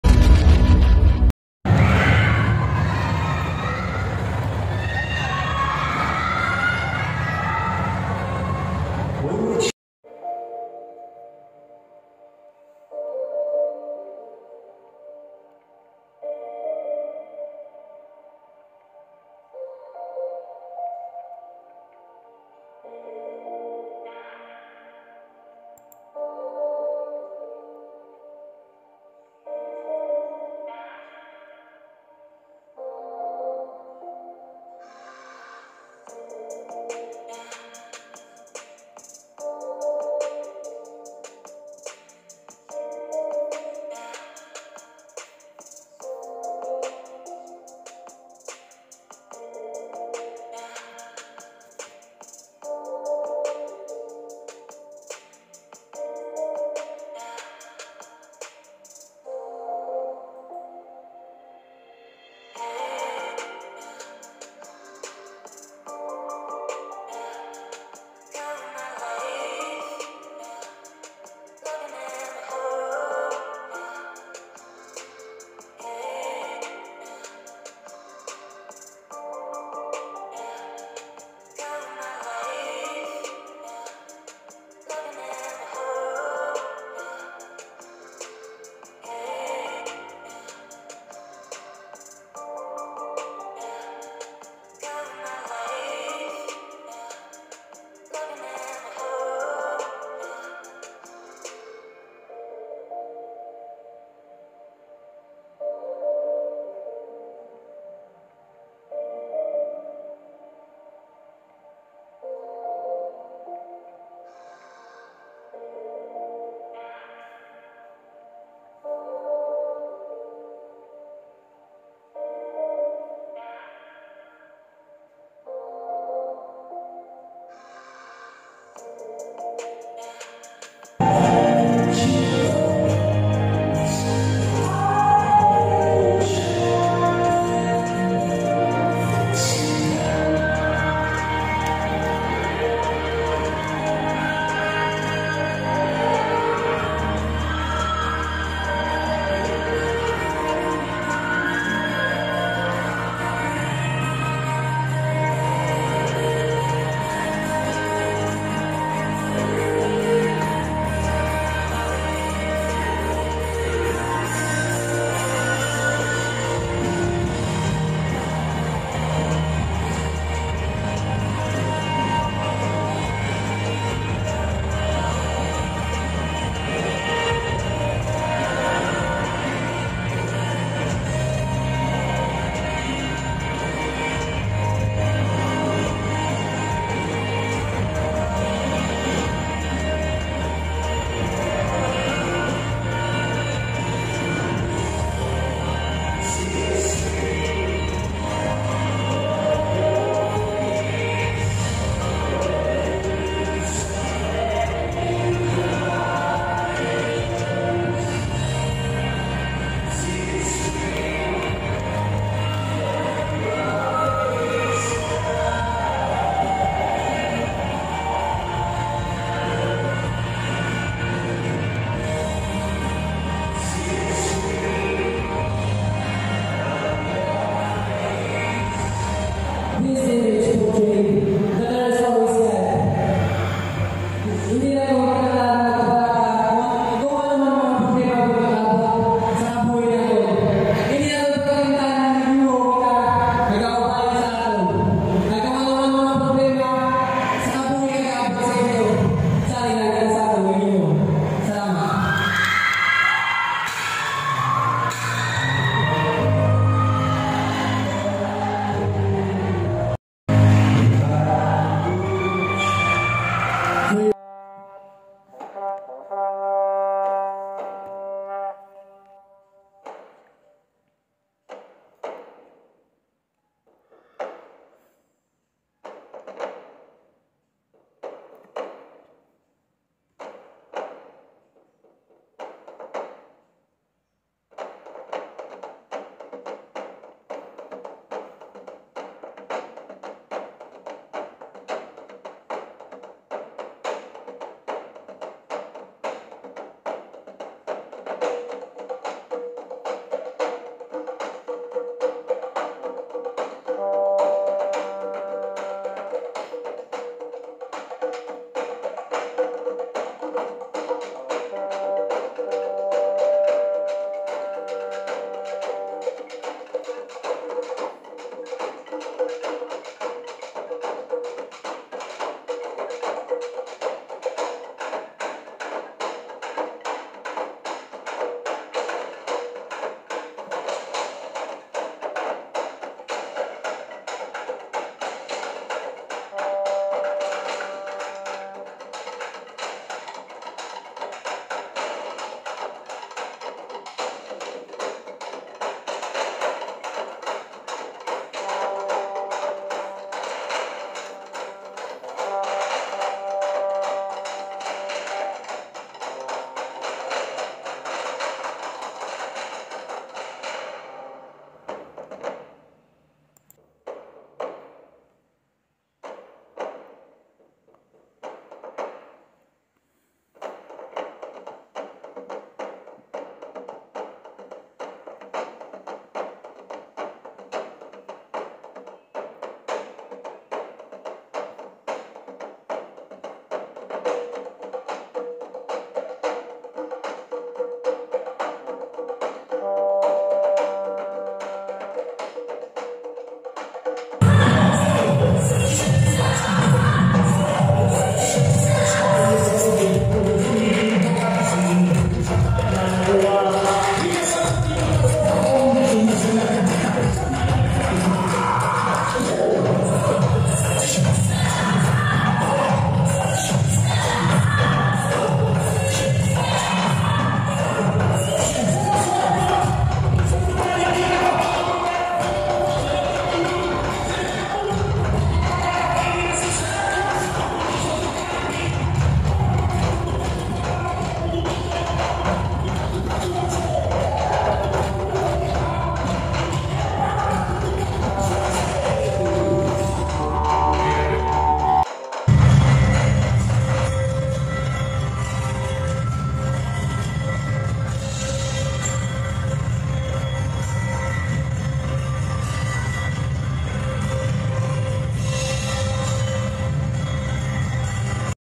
MR. TIGBAUAN I TALENT SHOW
Mr. Tigbauan contestants showcase their talent category.

Music
Credits: